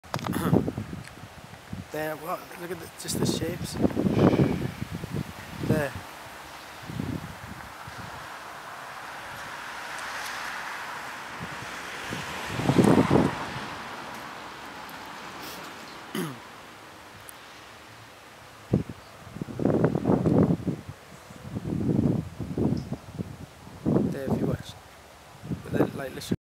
swarm of birds.avi
Swarm of birds above Fresnay l'Eveque (28) France.